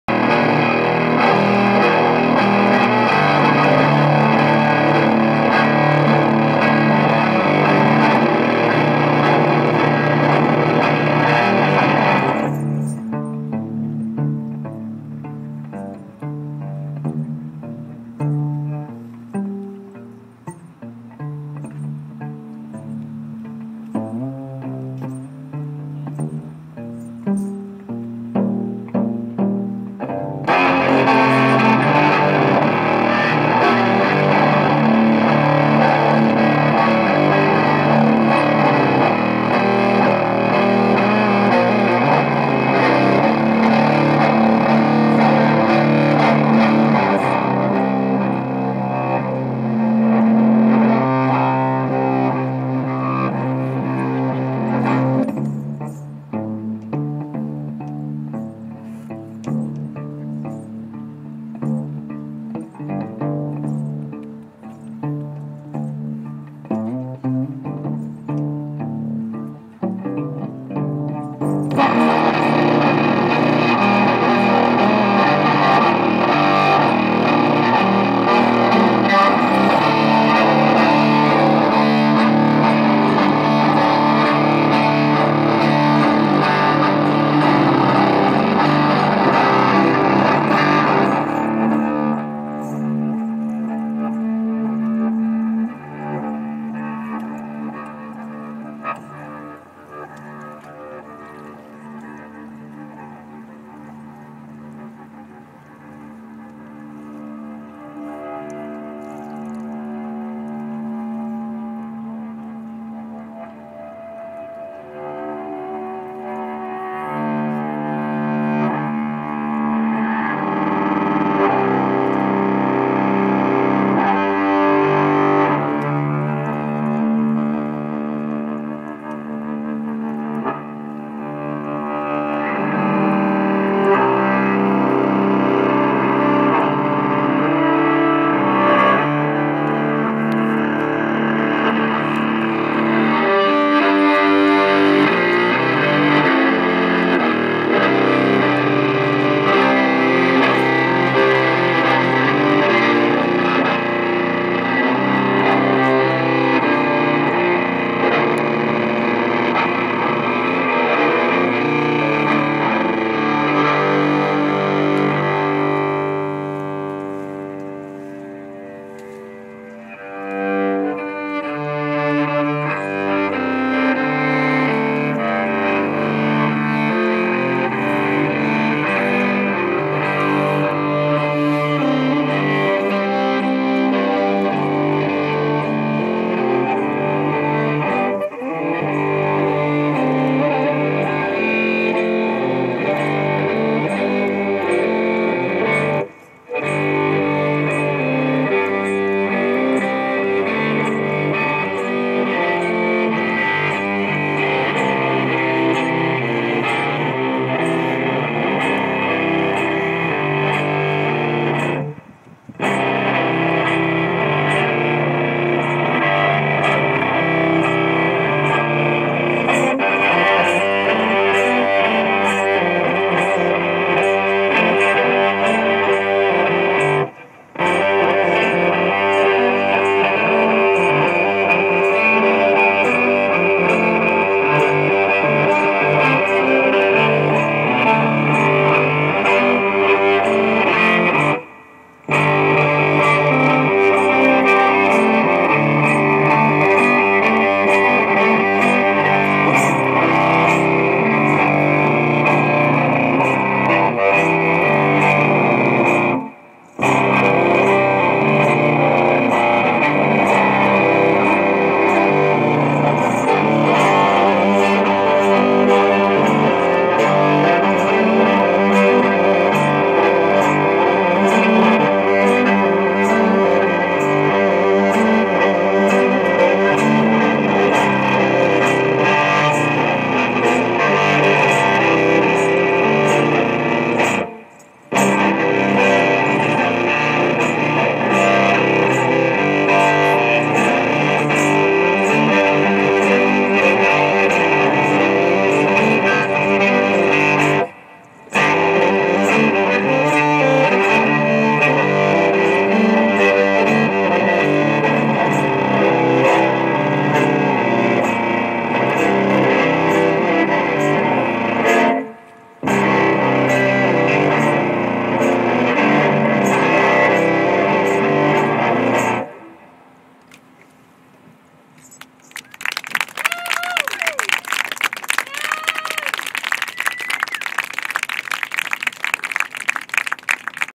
Lori Goldston at Dock Rock 09
The 3rd annual Dock Rock, featuring Lori Goldston, and the boats that passed by on this magical evening hosted by Erik Neumann.